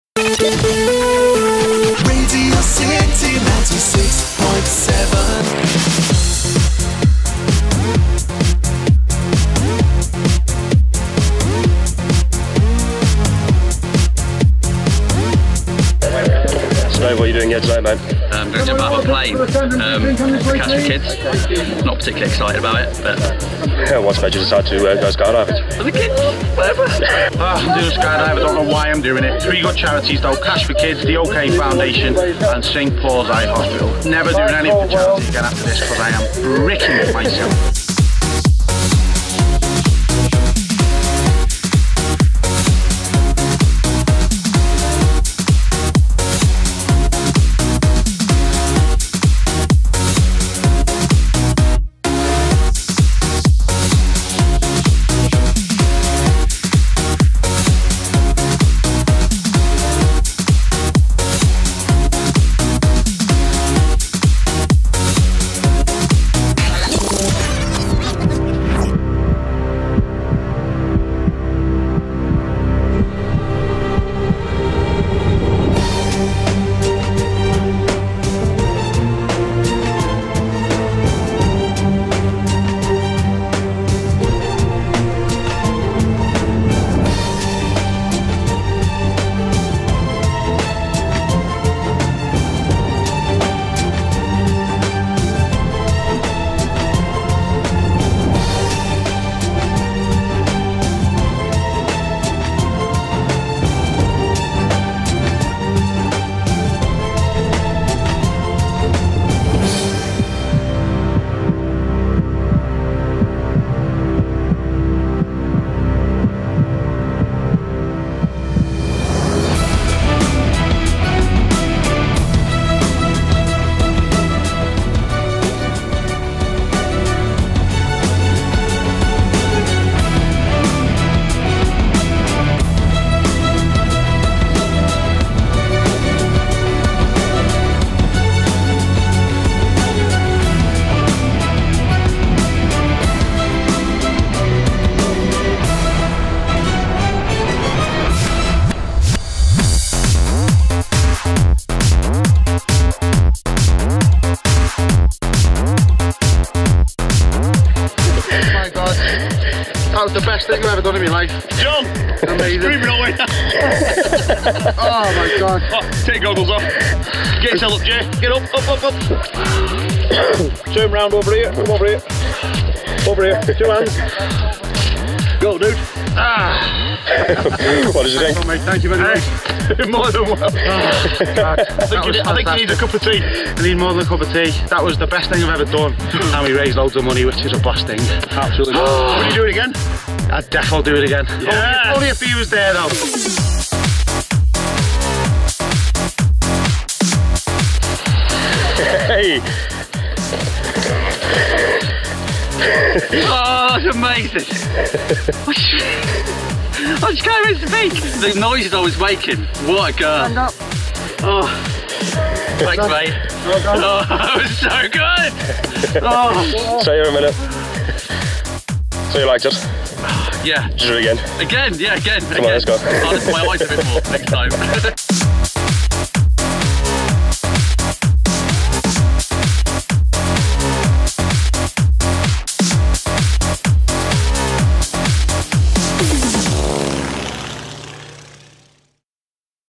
Dave and Jay's Charity Skydive
See Dave Kelly and Jay Hynd jump out of a perfectly good plane!  All in the name of charity...raising money for Cash for Kids, The OK Foundation and St Paul's Eye Hospital.